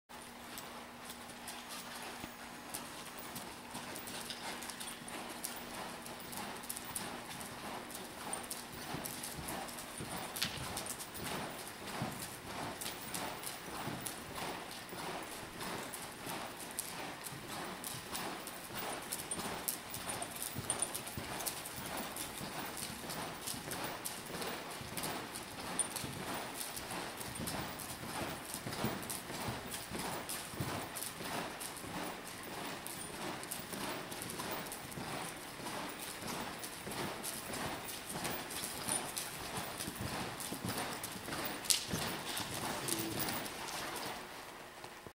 Wicked Cool loping right
Chip has a great start on him and is ready to be finished. He is 3 and has 90 days of riding time. He is stout, strong and will make a fabulous horse for a non-pro or youth. Chip is quiet, and will load, bathe and clip without issues. Very willing, sweet gelding.  Will continue to be in training until sold.